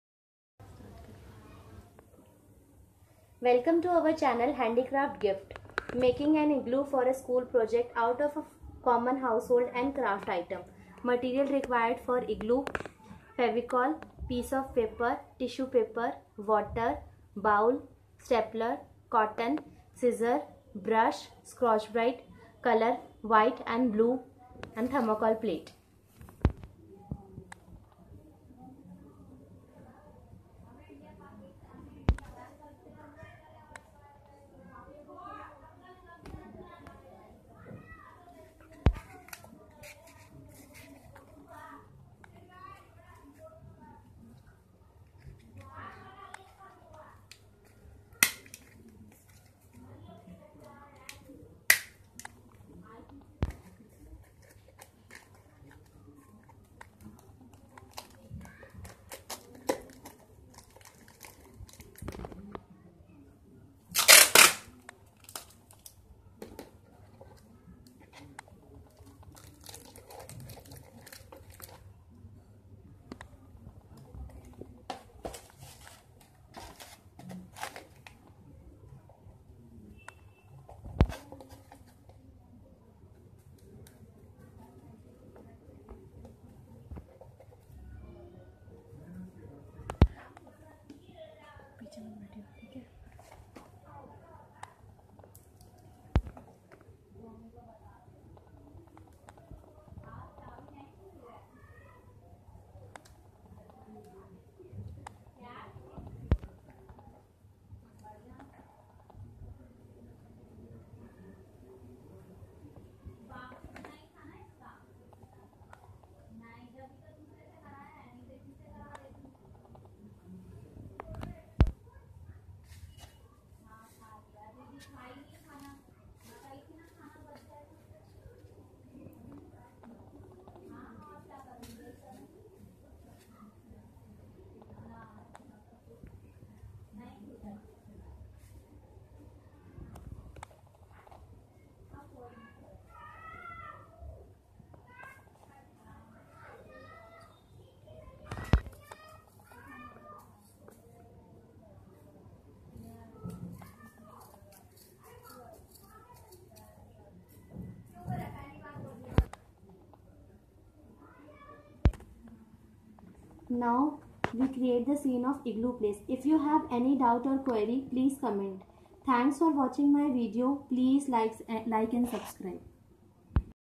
How to make a 3D model of an Igloo.
school project of igloo